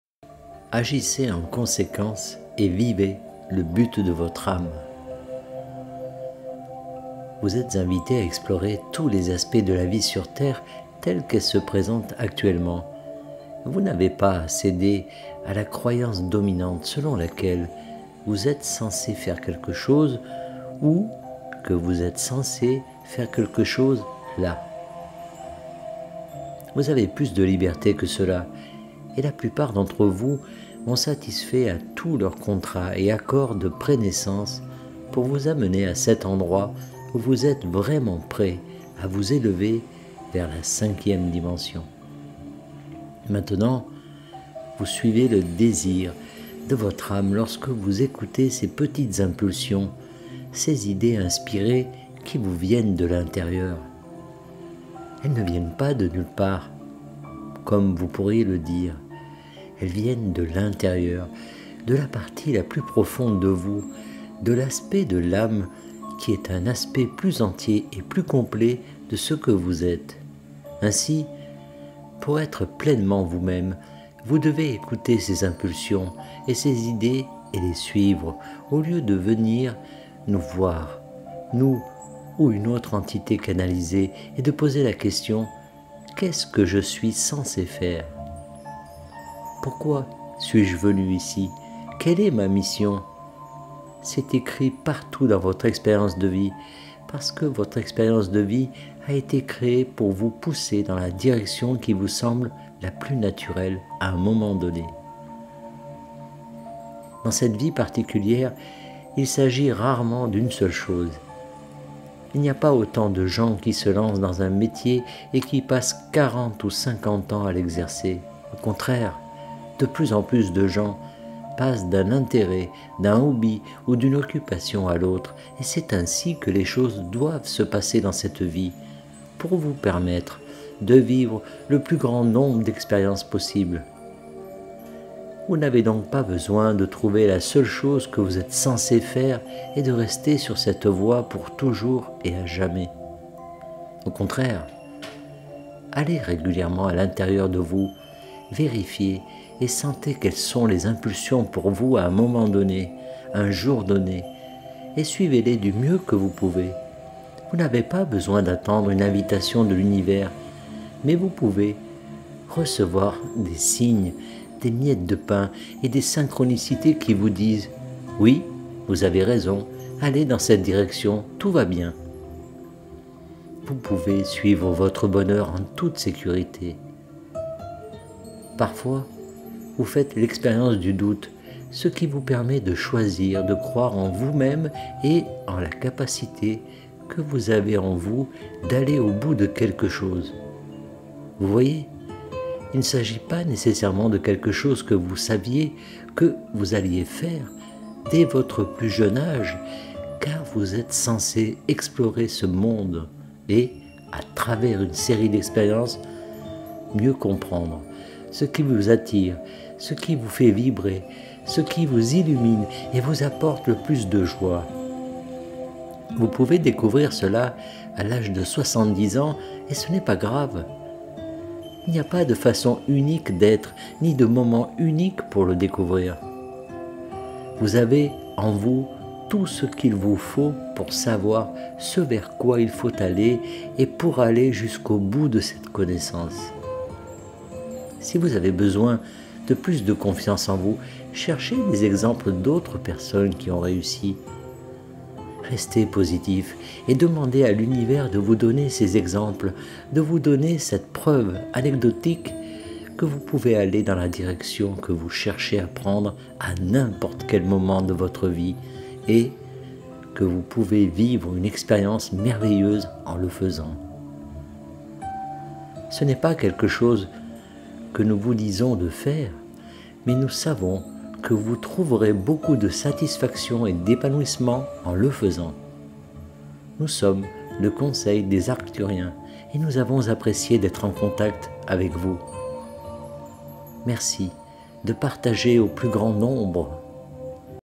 Agissez en conséquence et vivez le but de votre âme
Vous êtes invités à explorer tous les aspects de la vie sur Terre telle qu’elle se présente actuellement. Vous n’avez pas à céder à la croyance dominante selon laquelle vous êtes censés faire quelque chose ou que vous êtes censés faire quelque chose là.